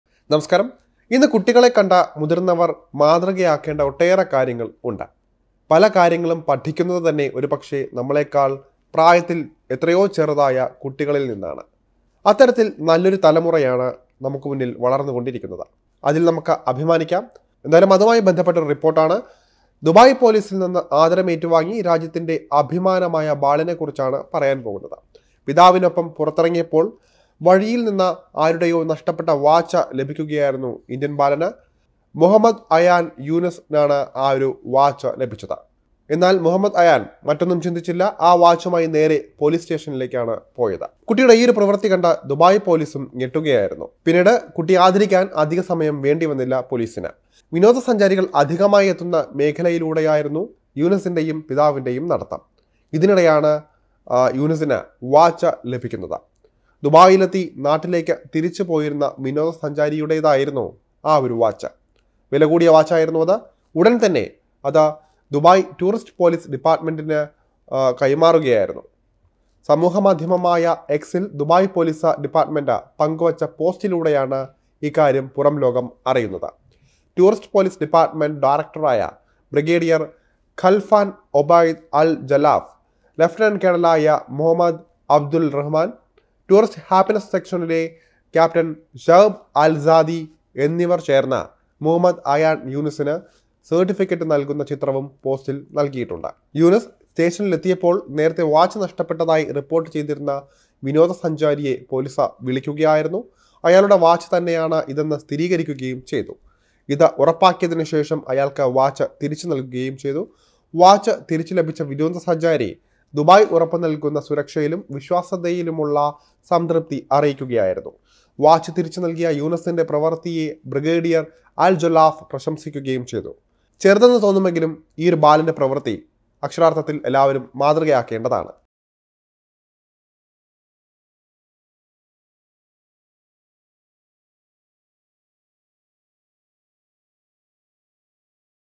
ദുബായ് പോലീസിൻ്റെ കയ്യടി നേടി മുഹമ്മദ് അയാൻ യൂനിസ് | Gulf News
ഇന്ത്യൻ ബാലന്
ദുബായ് പോലീസിൻ്റെ
ആദരം